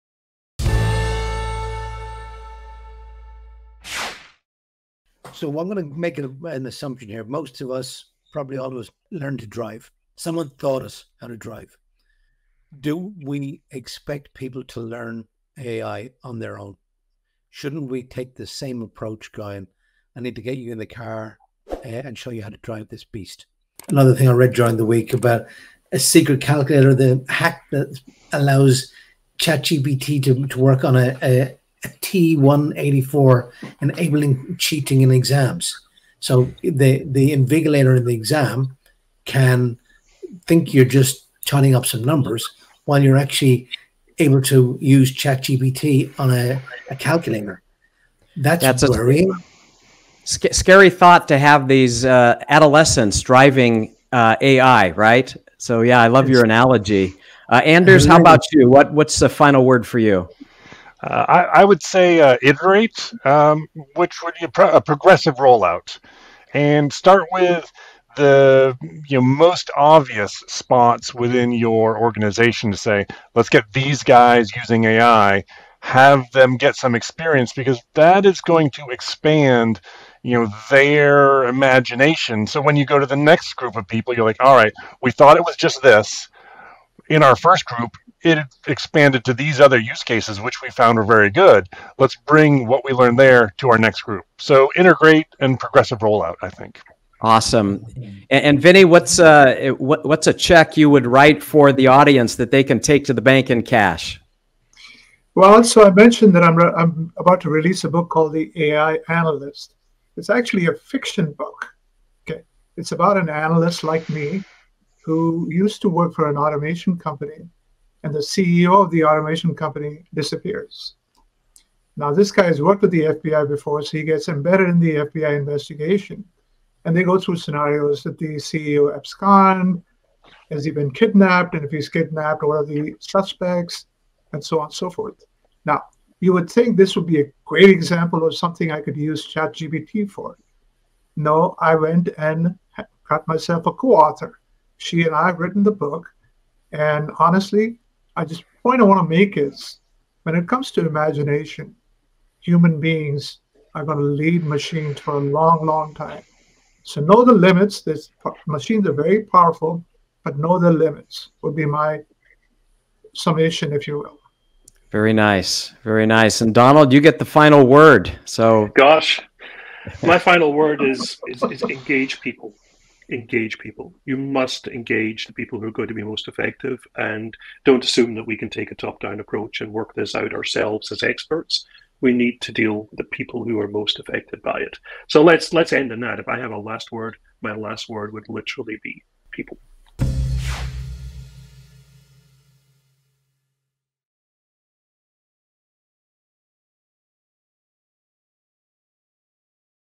AI Upskilling Advice for Data Leaders
Join the Insight Jam today!

How should organizations engage people in AI initiatives?

Our expert panel share their advice for anyone looking to get their people using AI. They touch on concerns about AI misuse in education, the importance of iterative implementation, the continued importance of human creativity, and the necessity of engaging people in AI initiatives.

This is a key takeaway from the Insight Jam 2024 Q3 Mini Jam panel, "Bridging the AI Divide: Upskilling Strategies for Enterprise Software Teams".

Panelists:
John Santaferraro - CEO & Founder - @FerraroConsulting
Anders Green - Founder - @AloxERP
Donald Farmer - Principal - TreeHive Strategy
Paul Byrne - SAP Programme Director - Dragon ERP
Vinnie Mirchandani - Chief Executive Officer - @dealarchitectinc6419

00:00 Paul's advice on getting behind the wheel
00:36 Anders on iterative AI implementation
01:45 Vinny's perspective on AI limitations
02:36 Donald's emphasis on people engagement